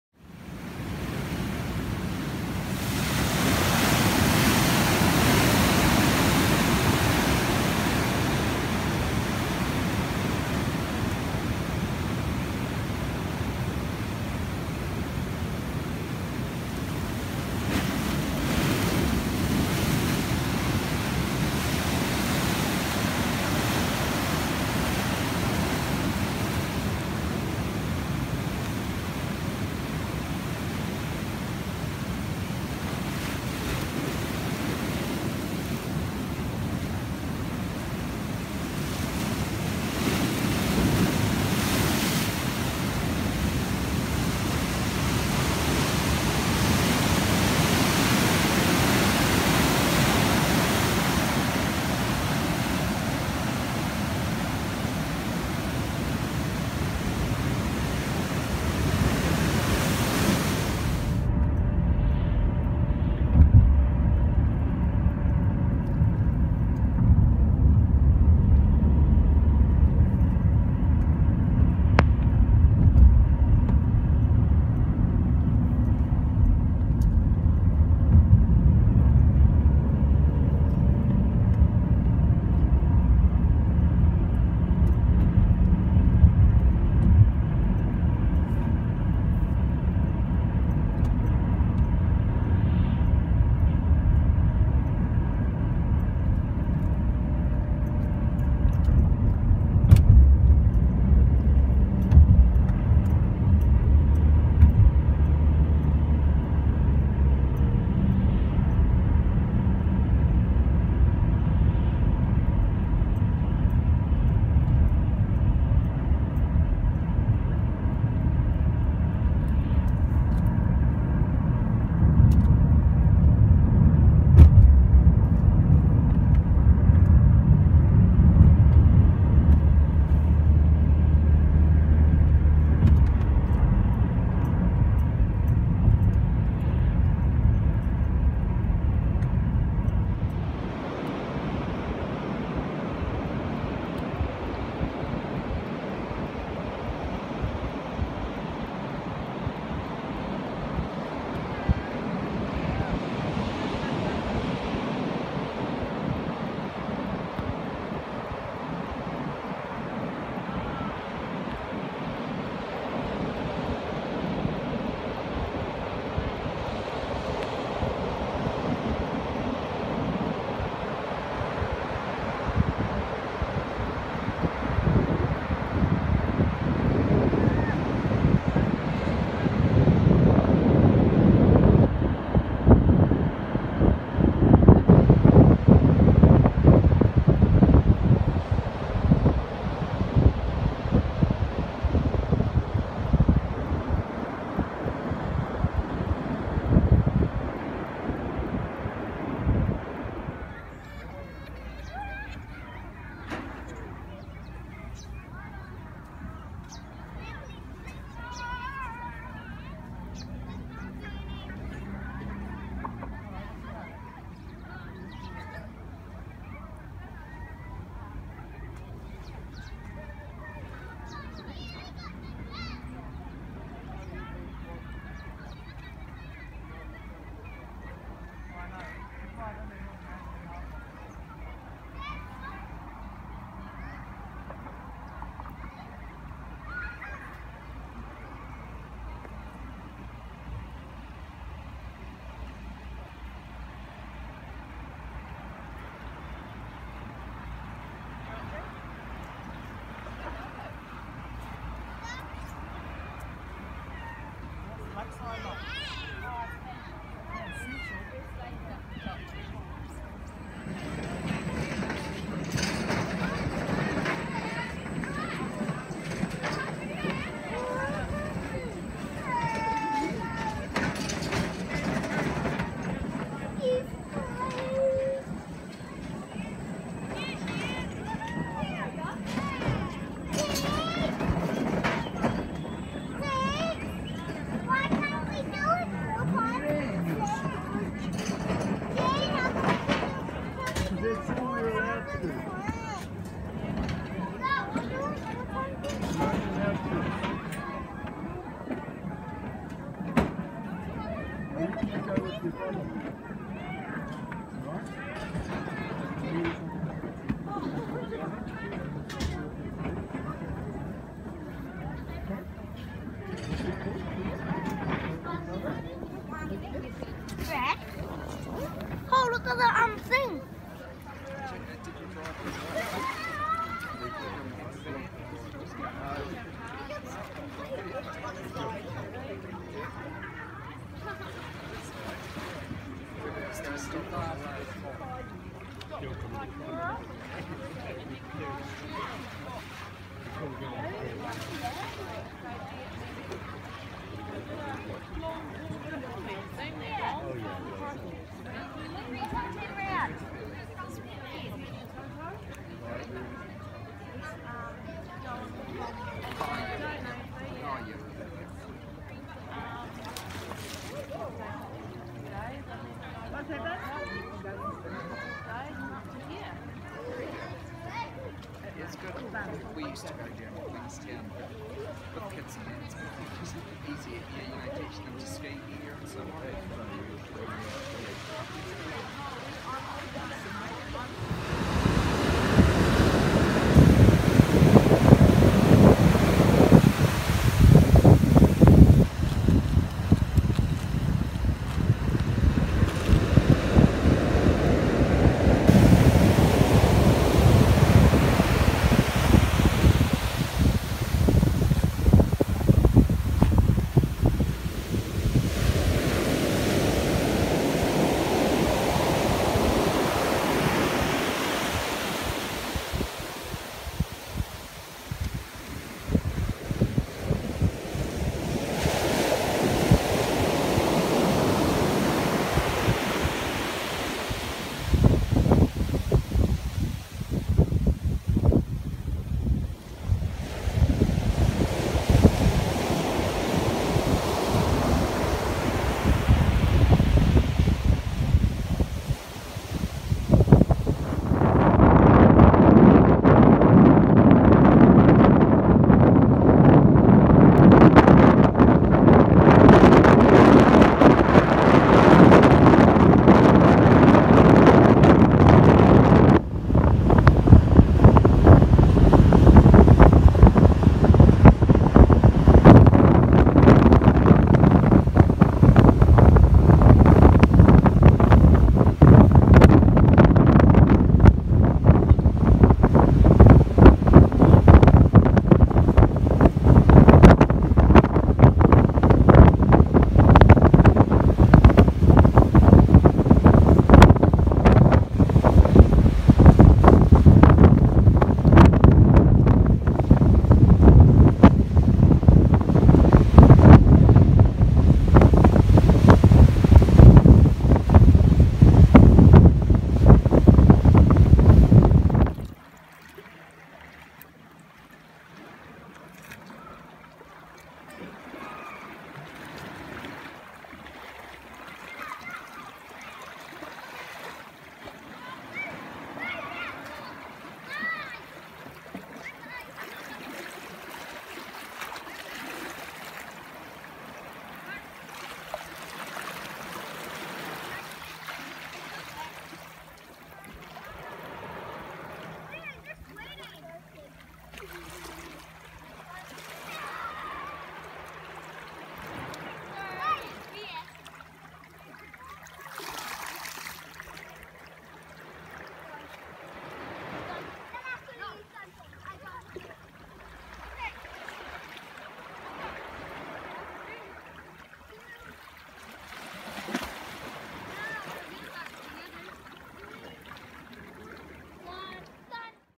Scenes from New Zealand (July 2016)
some footage from my recent holiday back in New Zealand ... Dunedin, Wanaka, Queenstown, Tekapo, Kaikoura and Kaiteriteri ... I miss the place dearly